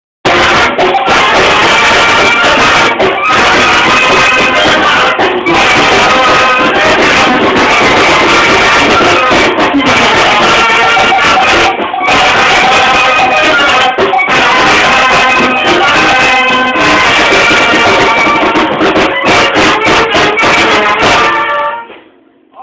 Banda la serranita de huauchinango puebla antes ilusion
tocando una cumbia chida en venta grande puebla